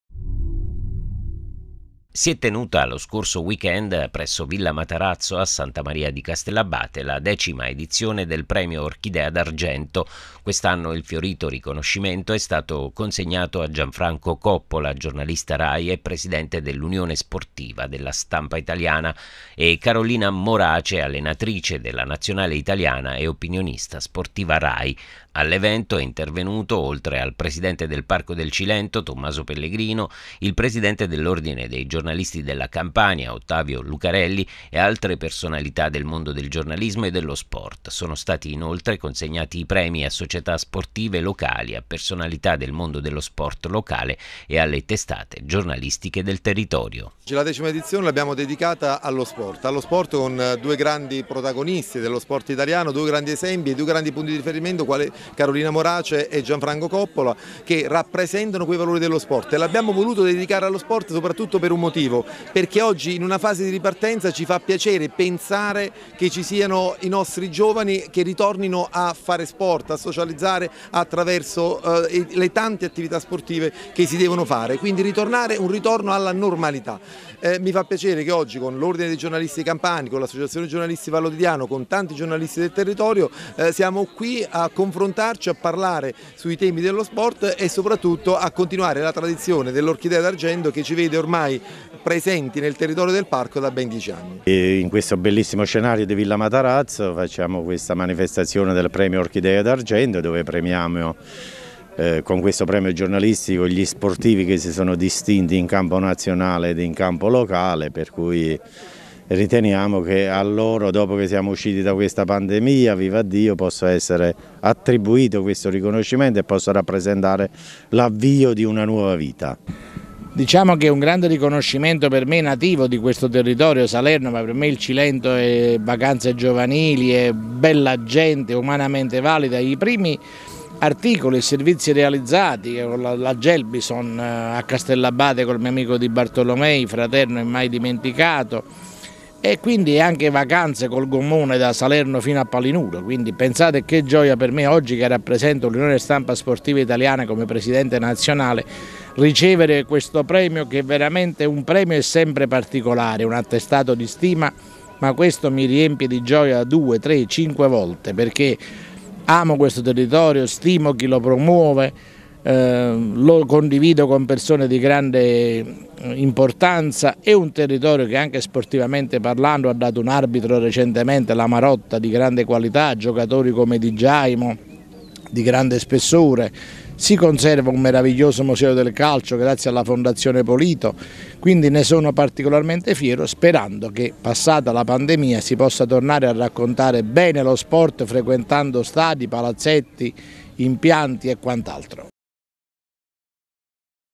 Castellabate, Premio giornalistico Orchidea d’argento X edizione 2021
Si è tenuta lo scorso sabato presso   Villa Matarazzo a Santa Maria di Castellabate
 la X ed. del Premio "Orchidea d'Argento". Quest'anno il "fiorito" riconoscimento è stato consegnato a Gianfranco Coppola, giornalista Rai e Presidente dell'Unione Stampa Sportiva Italiana (USSI) e a Carolina Morace, allenatrice della Nazionale Italiana e opinionista sportiva Rai.